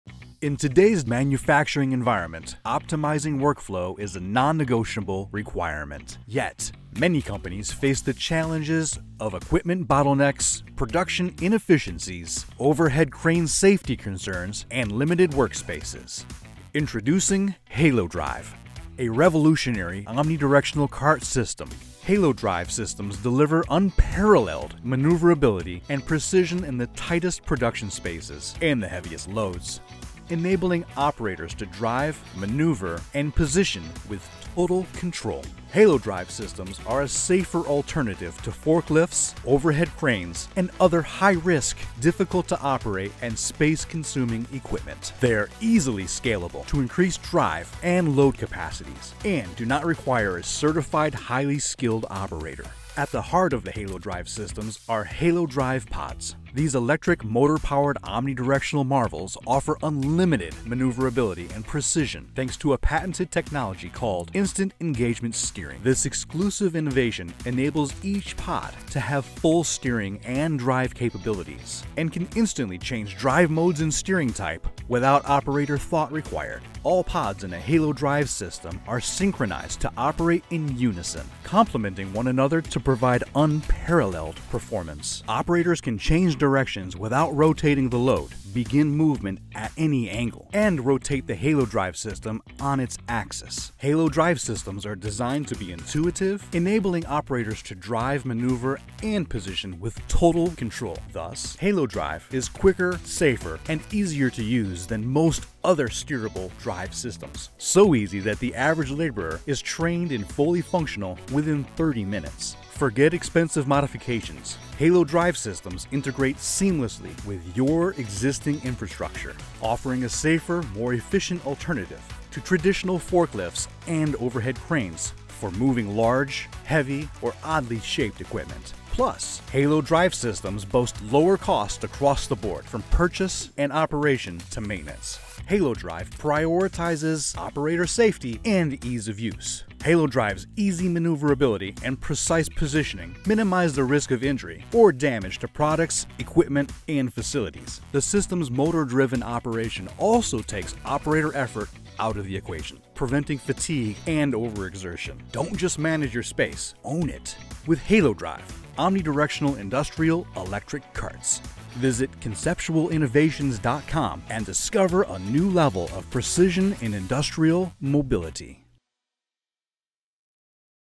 HaloDrive™ System: Industrial Omnidirectional Wheels
Introducing a revolutionary industrial omnidirectional wheel that delivers unparalleled maneuverability and precision in the tightest production spaces... and the heaviest loads, enabling operators to drive, maneuver, and position with total control. HaloDrive™ Pods™ are designed for modular integration to add mobility into a variety of extreme-duty industrial applications. Thus, capable of essentially moving limitless weight and size capacity. simply increase Pods to accommodate the application’s requirements.

Conceptual Innovations’ patented technology featured in the HaloDrive™ Pods™ enables these motor-powered, omnidirectional wheels unlimited maneuverability with extreme precision (+/- 1/4” Variance). Each of the HaloDrive Pods™ is independently steered and driven enabling unrestricted movement in any direction, at any angle. Pods can instantly switch drive modes, making it easy to control, maneuver, and position.

Or visit us online to find out more about our HaloDrive™ System:
utm_source=halodrive_systems_video&utm_medium=video&utm_campaign=halodrive

0:01  Why HaloDrive™ ?
0:15  Introduction to HaloDrive™
0:22  Unparalleled maneuverability and precision
0:30  Pinpoint Accuracy
0:36  Safer Alternative
0:45  Easily Scalable
0:49  Minimal Skill Operation
0:53  HaloDrive™ Pods™
1:03  Instant Engagement Steering
1:17  Synced for precision
1:25  Omnidirectional Movements
1:34  Intuitive Design
2:09  Lower Costs
2:15  Prioritizes Operator Safety
2:31  Prevents Overexertion
2:36  Own it!